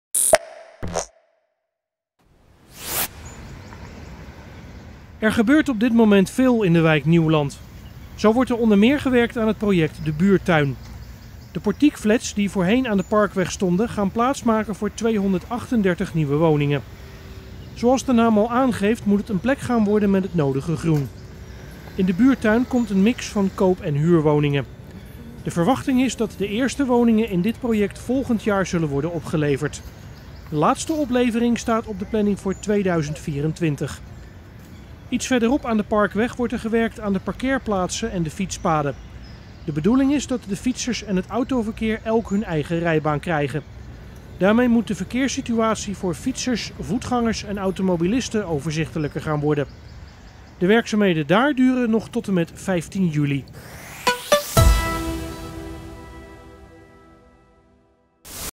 SCHIE TV: Sloop en wegwerkzaamheden in Nieuwland
Er gebeurt op dit moment veel in de wijk Nieuwland. Zo wordt er onder meer gewerkt aan het project De Buurttuin. De portiekflats die voorheen aan de Parkweg stonden gaan plaats maken voor 238 nieuwe woningen.

Zoals de naam al aangeeft moet het een plek gaan worden met het nodige groen. In De Buurttuin komt een mix van koop- en huurwoningen. De verwachting is dat de eerste woningen in dit project volgend jaar zullen worden opgeleverd. De laatste oplevering staat op de planning voor 2024.

Iets verderop aan de Parkweg wordt er gewerkt aan de parkeerplaatsen en de fietspaden. De bedoeling is dat de fietsers en het autoverkeer elk hun eigen rijbaan krijgen. Daarmee moet de verkeerssituatie voor fietsers, voetgangers en automobilisten overzichtelijker gaan worden. De werkzaamheden duren nog tot en met 15 juli.